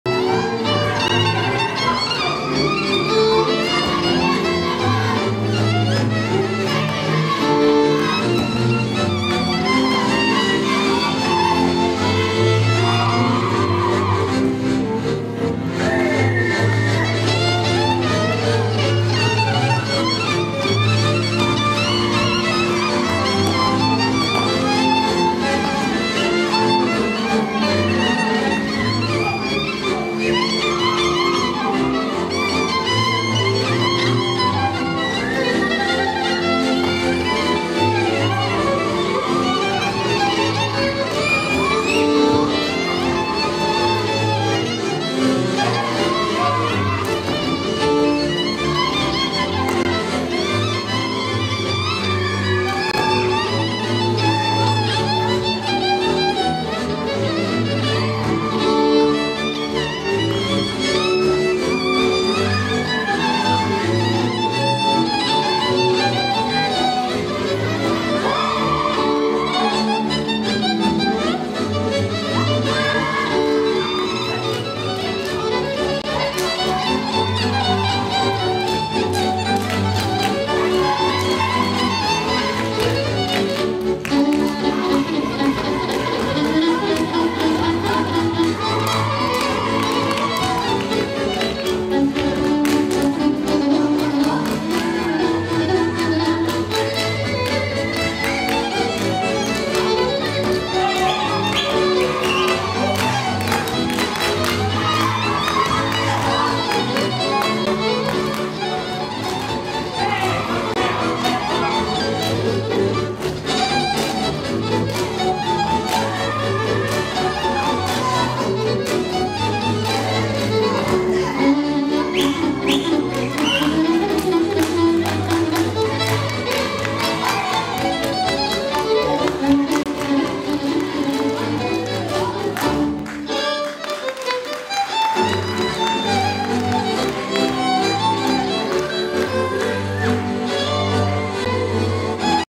Kis-Kökényes Néptánccsoport: Györgyfalvi táncok
V. "Kiöntött a Kis-Küküllő" nemzetközi néptánctalálkozó
Dicsőszentmárton, 2011. november 5.